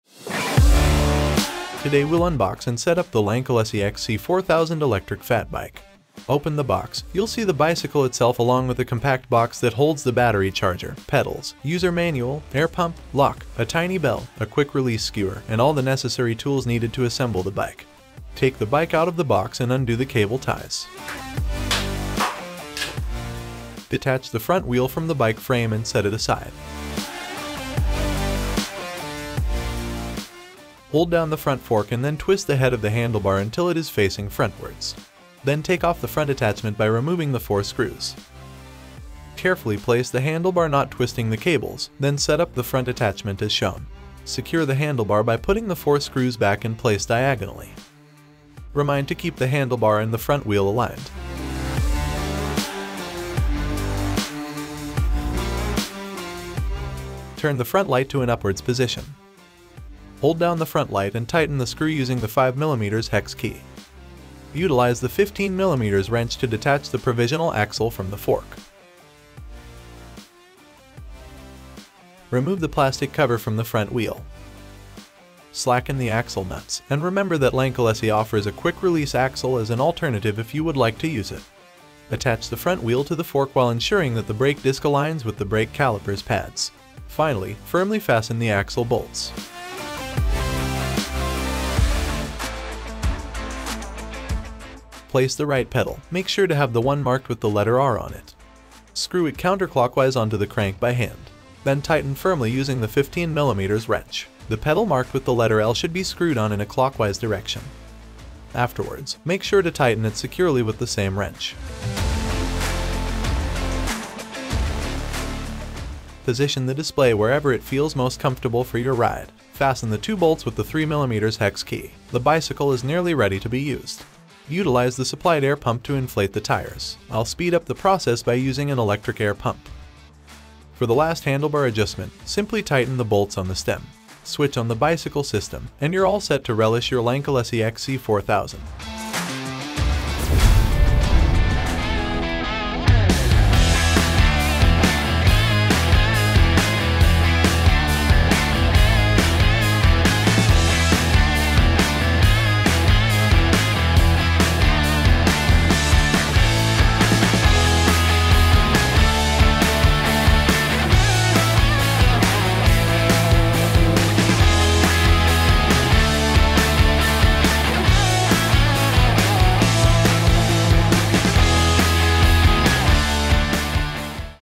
Lankeleisi XC4000 Electric Fat Bike Unboxing and Assemble: A Step-by-Step Guide
Learn how to unbox and assemble your new Lankeleisi XC4000 Electric Fat Bike in this step-by-step guide. Our video walks you through the entire process, from removing the bike from its packaging to installing the battery and adjusting the brakes. Even if you've never assembled a bike before, you'll be able to follow along with our simple instructions and clear visuals.

By the end of this video, you'll have your new electric fat bike fully assembled and ready to ride. So grab your bike and let's go!

Once you have completed these steps, your Lankeleisi XC4000 Electric Fat Bike will be fully assembled and ready to ride. Have fun!

Chapters:
00:00 - Intro
00:05 - Unboxing the Lankeleisi XC4000 E-Bike
00:27 - Set up the handlebar
00:54 - Assemble the Front Wheel
01:32 - Assemble the pedals
01:52 - Setup the Display and tires
02:20 - Lankeleisi XC4000 Electric Fat Bike Test Ride